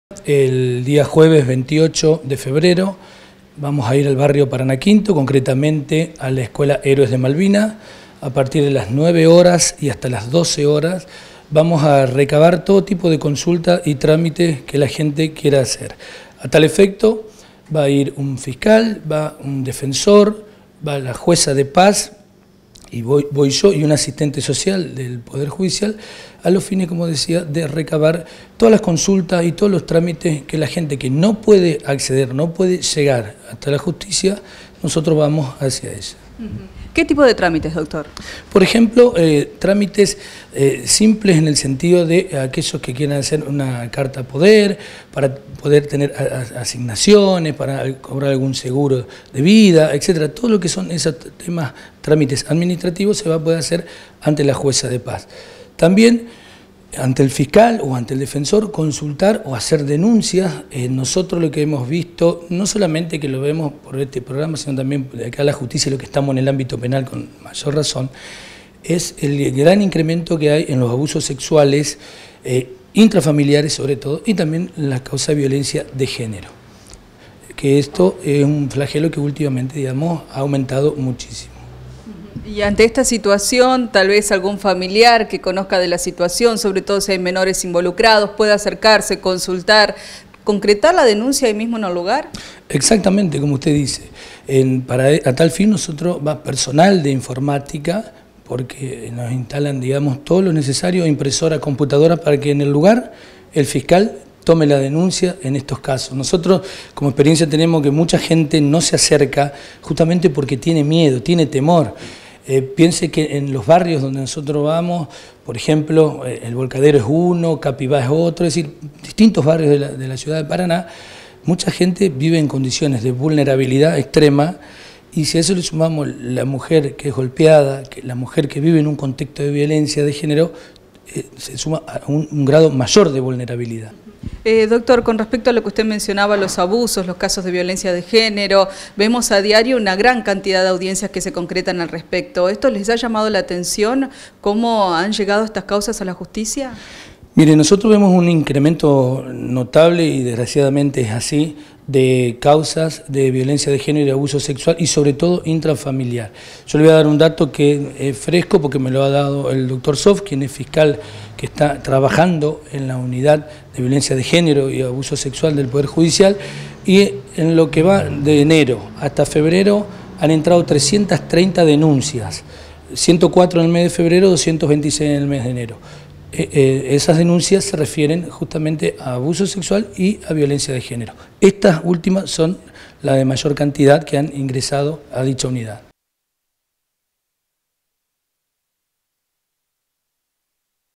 JUSTICIA VA A LOS BARRIOS
La Justicia va a los barrios: Recibirán consultas y denuncias en Paraná V
Será jueves 28 de 9 a 12 en la Escuela Héroes de Malvinas. "Muchos no se acercan hasta el Poder Judicial por miedo o temor, porque no se animan o no pueden llegar",